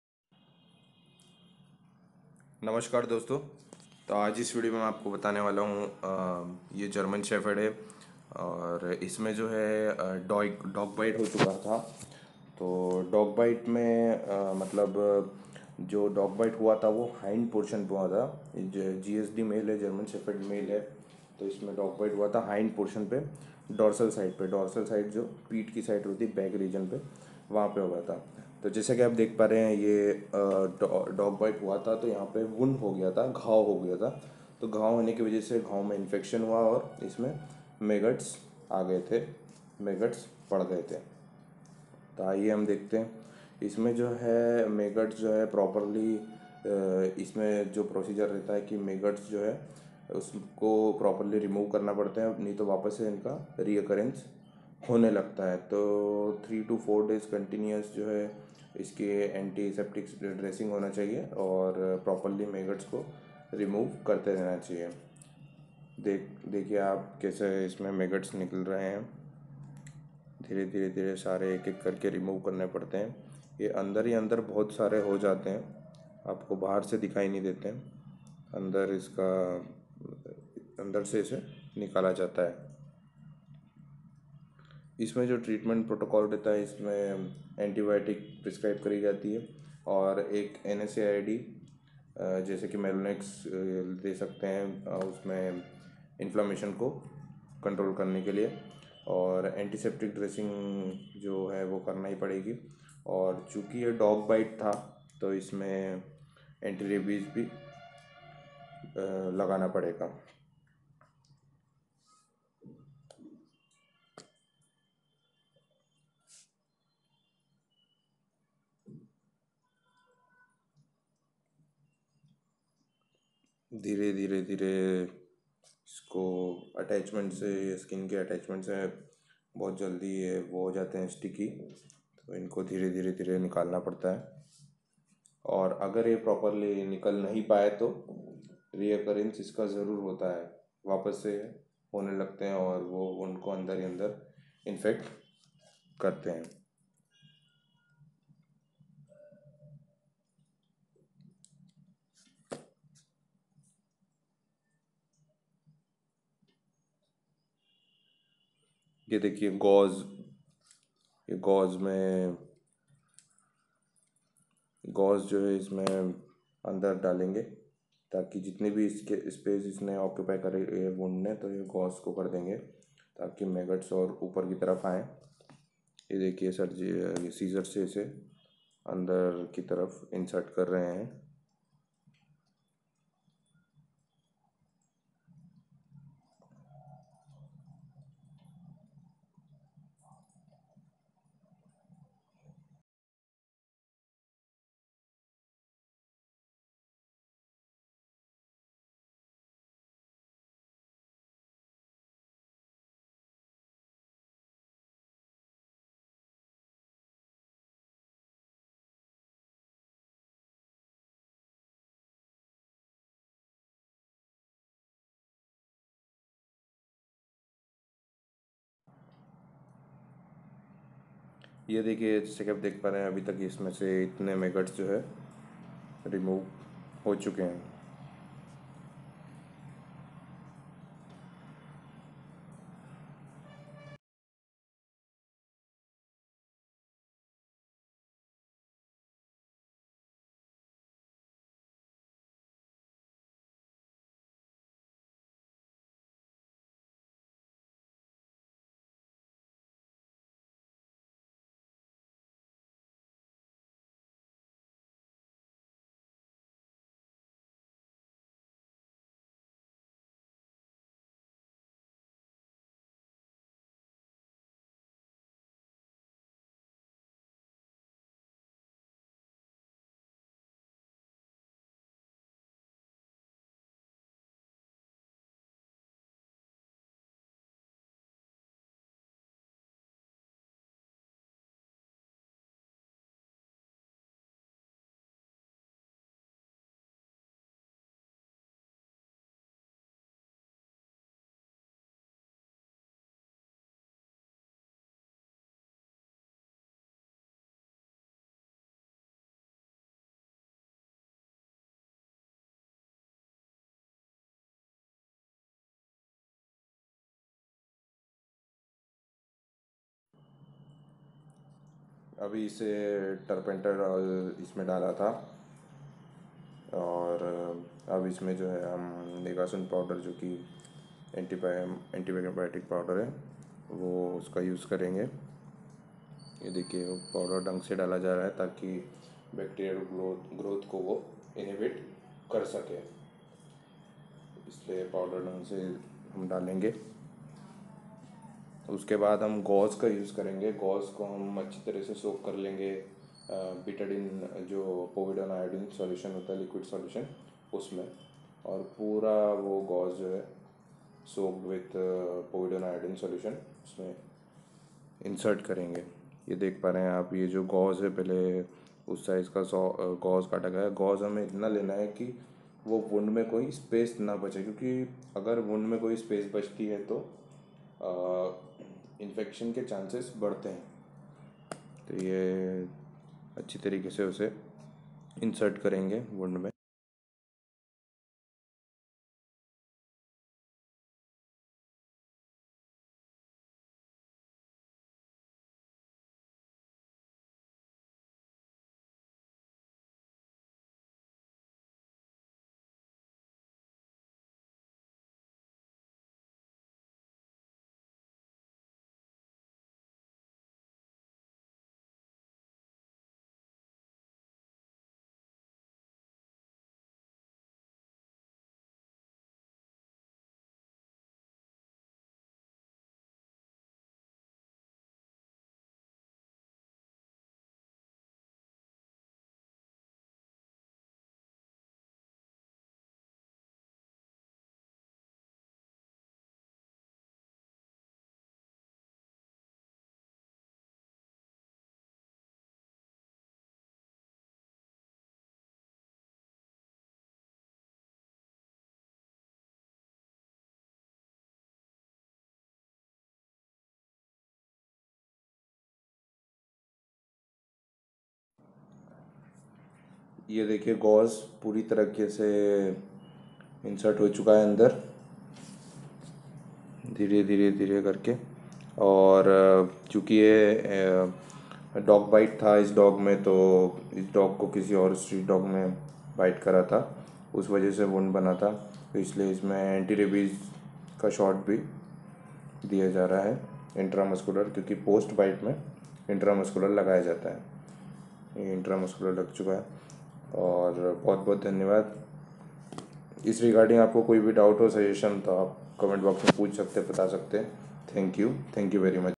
MAGGOT Wound | in German Shepherd | Dog bite case | full treatment protocol
Maggot wound
in German Shepherd
Dog bite
Full treatment for maggot wound is shown in this video.

A maggot is the larva of a fly (order Diptera); it is applied in particular to the larvae of Brachycera flies, such as houseflies, cheese flies, and blowflies, rather than larvae of the Nematocera, such as mosquitoes and crane flies.

As with fleas and ticks, maggots can be a threat to household pets and livestock, especially sheep. Flies reproduce rapidly in the summer months, and maggots can come in large numbers, creating a maggot infestation and a high risk of myiasis (a maggot infestation of living tissue) in sheep and other animals. Humans are not immune to the feeding habits of maggots and can also contract myiasis. Interaction between humans and maggots usually occurs near garbage cans, dead animals, rotten food and other breeding grounds for maggots.

When maggots turn into adult flies and start the life cycle over, numbers will grow exponentially if unchecked, but disease, natural predators and parasites keep the population under control. Sealing garbage and using a garbage disposal or freezing rotting leftovers until waste collection day helps prevent infestation. Introducing an environmental control, such as Hister beetles, can also help reduce maggot populations.